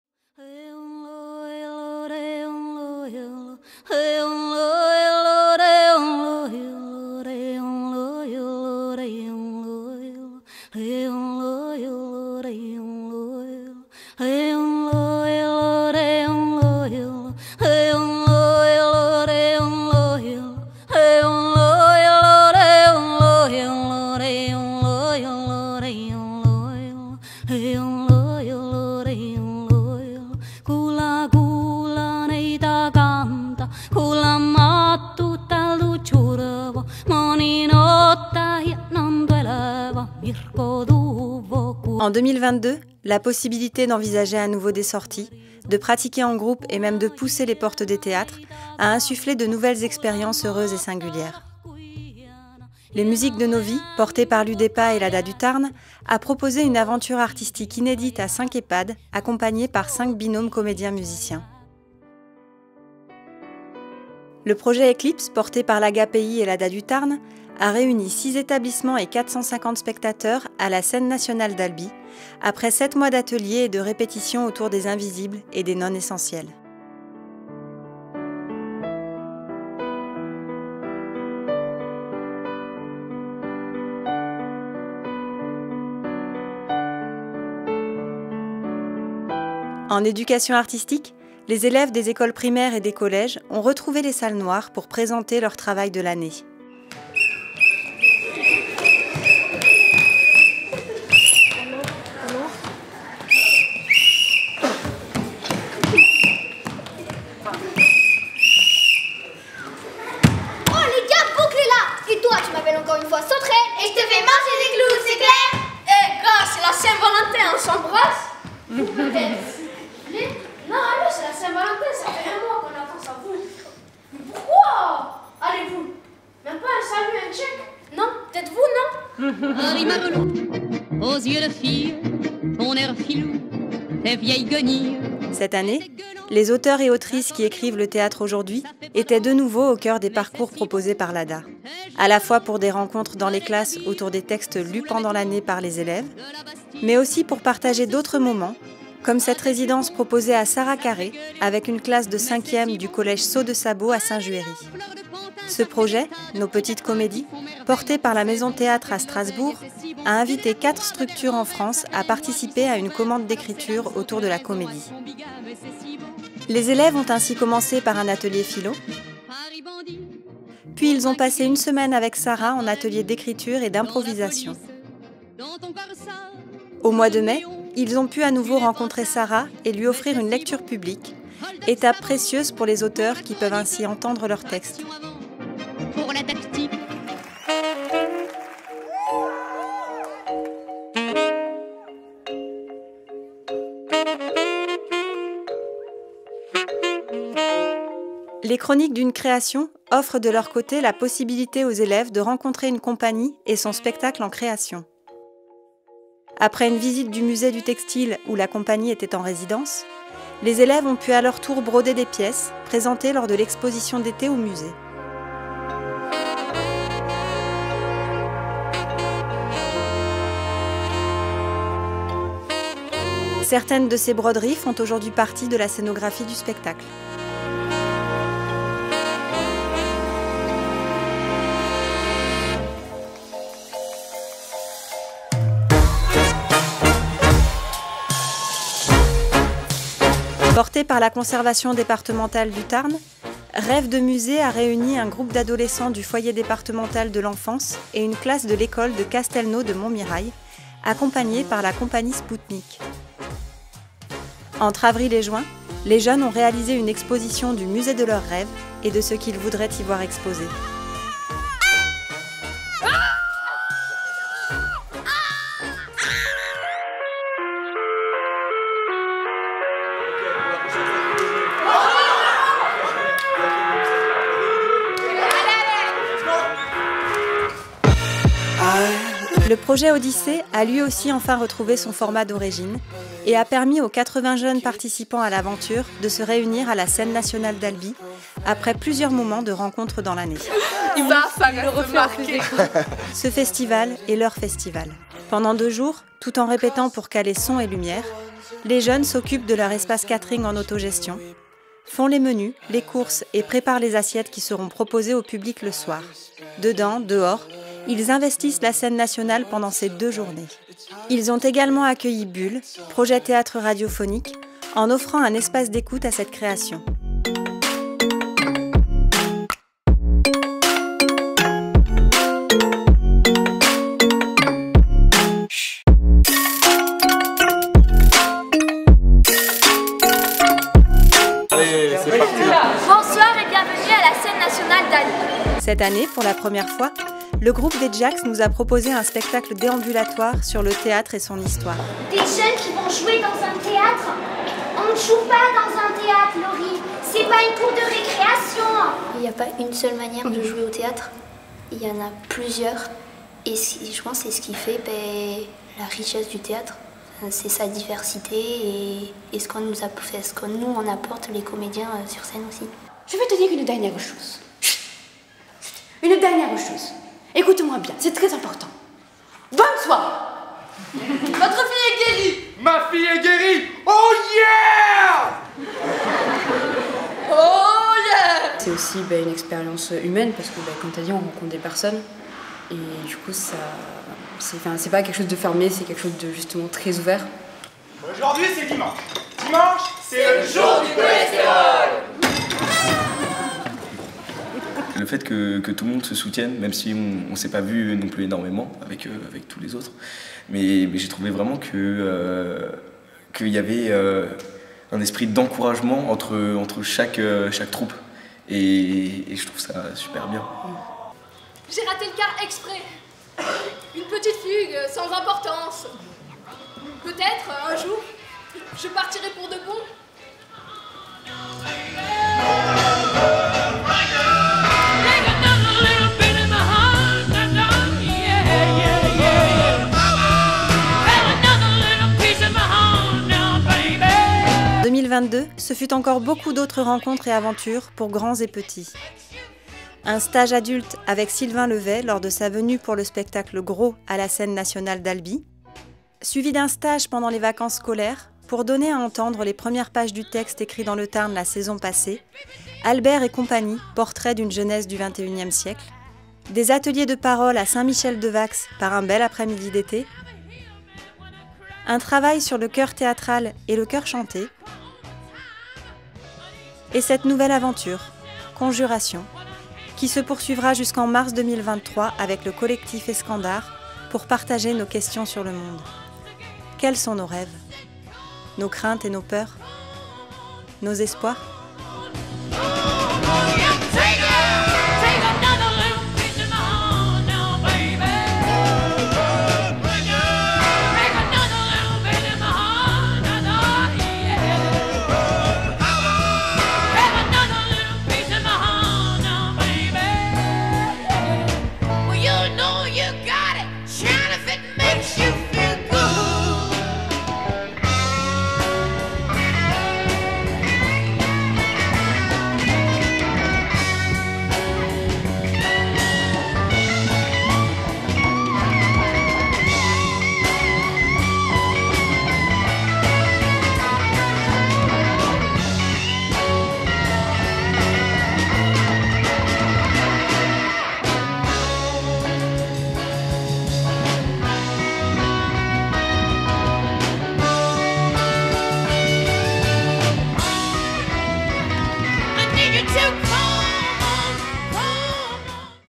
BILAN VIDÉO 2022 - THÉÂTRE
Chaque année, à l'occasion de l'Assemblée Générale de l'ADDA du Tarn, les chargées de missions de l'association réalisent des bilans vidéos présentant les projets et dispositifs phares de l'année passée.
Montage : Chrystel Rostaing de Es'Tampa